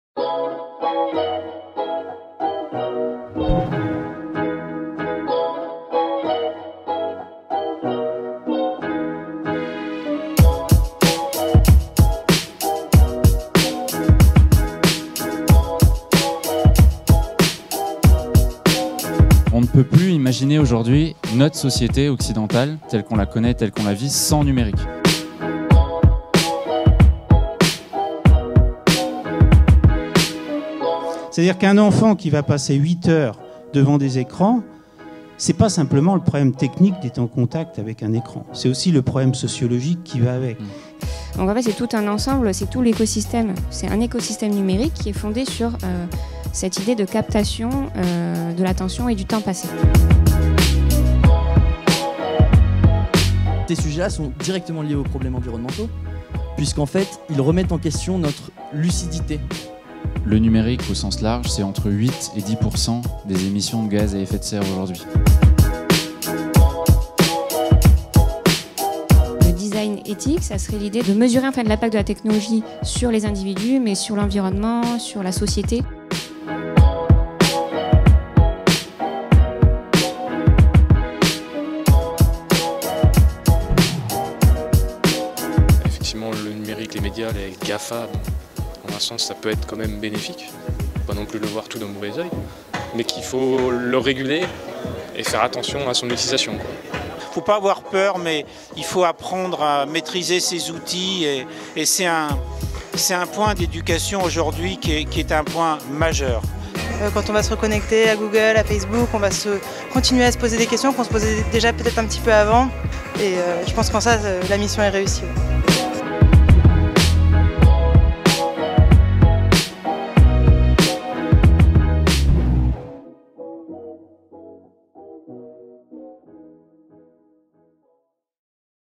Cap Sciences - Happy hour #4
Retour en images sur la soirée happy hour#4 avec Mellie Laroque, Tim Krief & Francis Eustache, avec pour thème "Géants du net : comment manipulent-ils nos cerveaux ?"